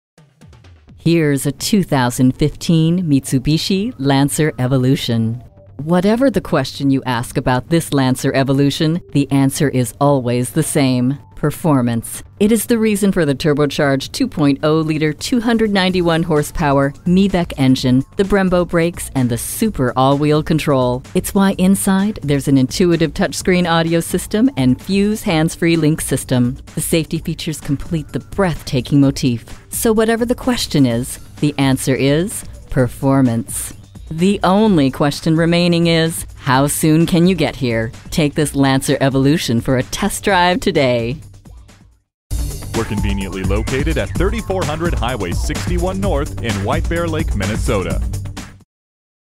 2015 Mitsubishi Lancer Evolution St-Paul White-Bear-Lake, MN #75922 - SOLD
Year: 2015
Make: Mitsubishi
Model: Lancer Evolution
Trim: 4dr Sdn Man GSR
Engine: 2.0 L 4 Cyl. Cyl.
Transmission: Manual
Color: Mercury Gray
Stock #: 75922
VIN: JA32W8FV2FU022915
Snag one of the Last EVO X GSRs left. Legendary Super All Wheel Control paired with a wicked 2.0 Turbo Motor will put a huge smile on your face. The bluetooth and touch screen audio system will keep the smile on your face as you pass other less dynamic vehicles! Ask about incoming Evolutions on their way from the factory. Don't miss your chance to own a part of history.

Address:
3400 Highway 61 N